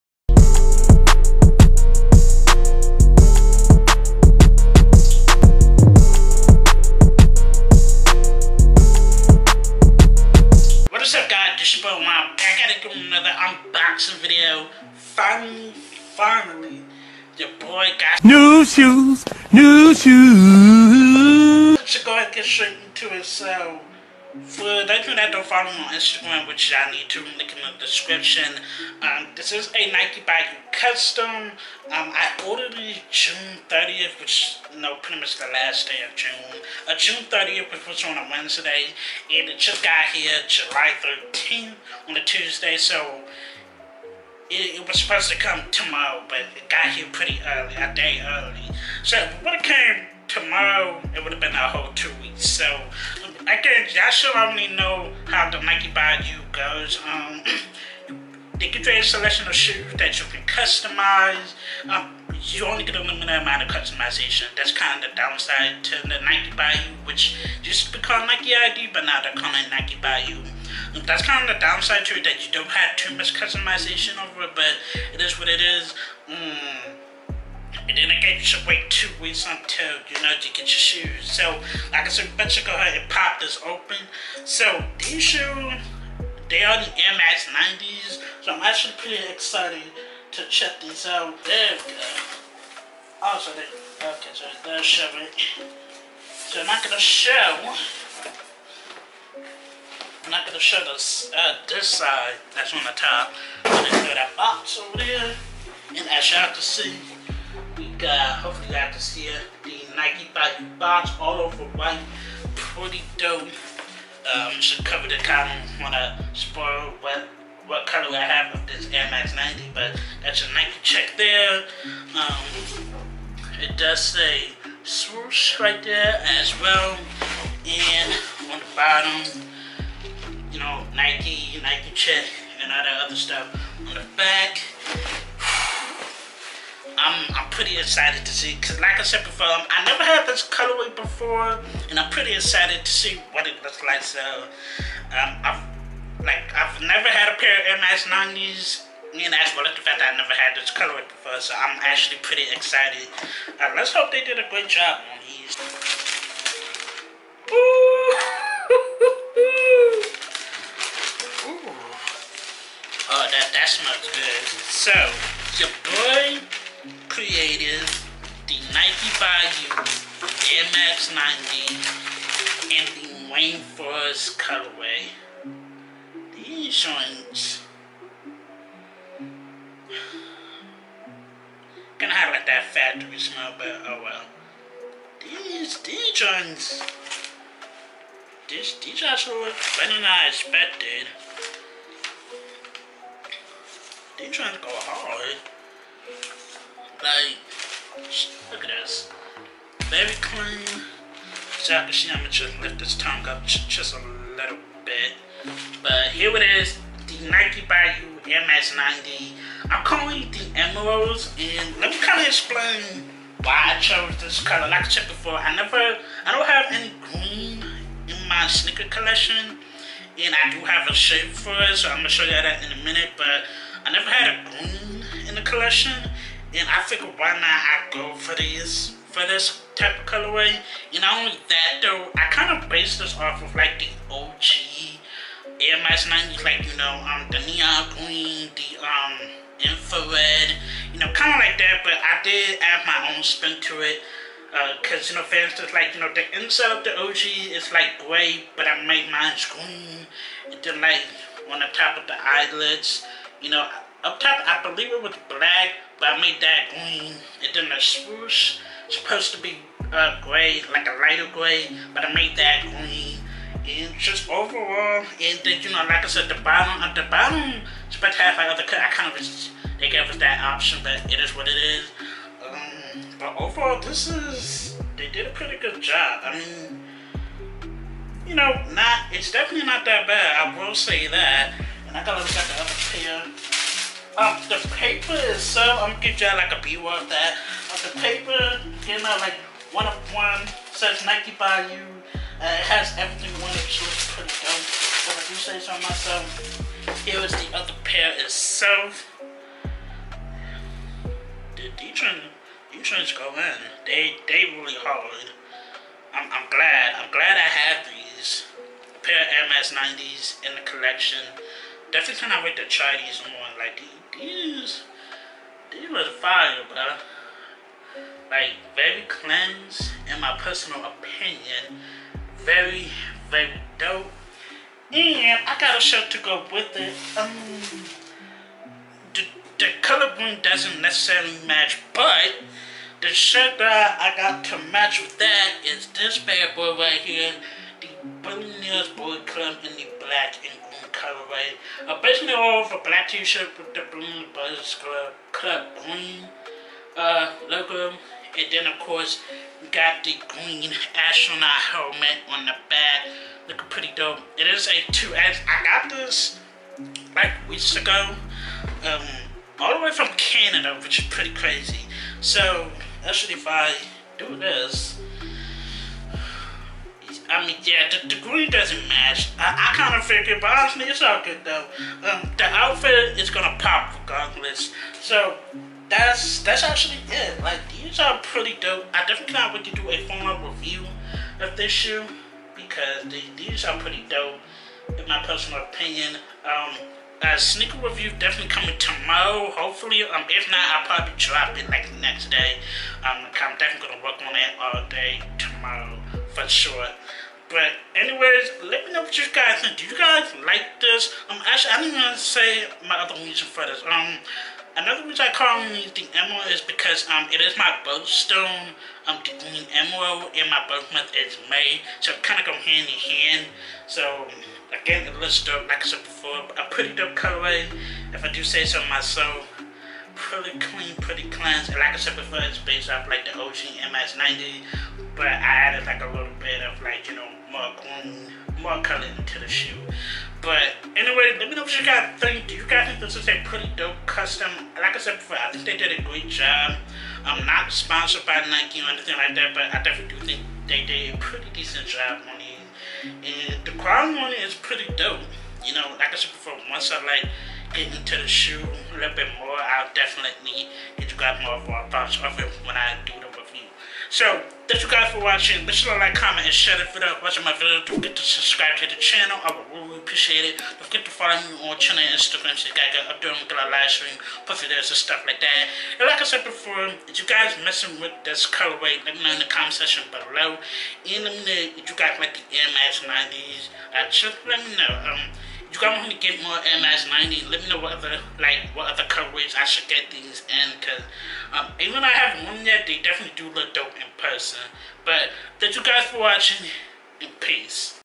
[MY FIRST CUSTOM SNEAKERS!!!] NIKE BY YOU: AIR MAX 90 UNBOXING!!!
In today's video, I will be unboxing a Nike By You sneaker that I customized using the Air Max 90 model. Be easy!!!